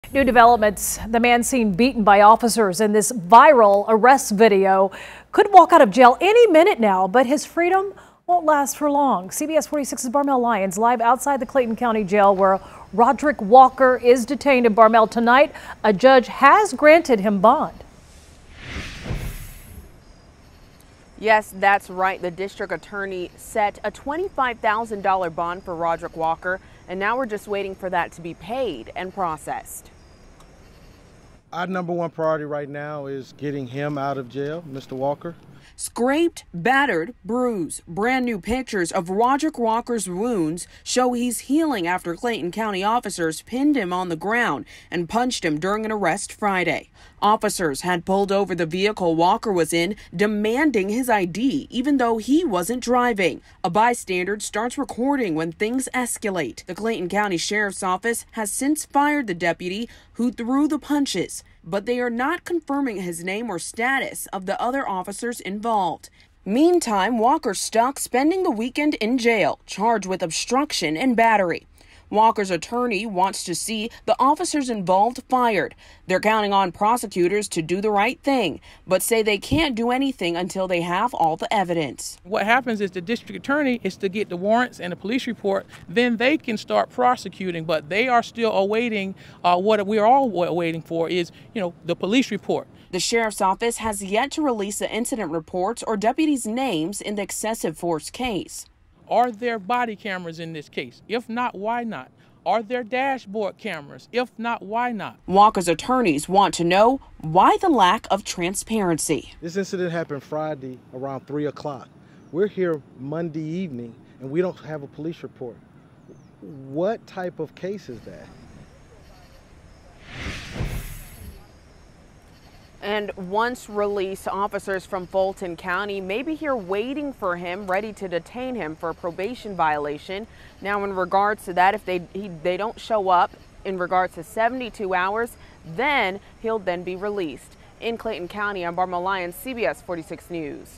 Victim in viral arrest video gets bond
Roderick Walker who was beaten by two Clayton County deputies get bond Monday night.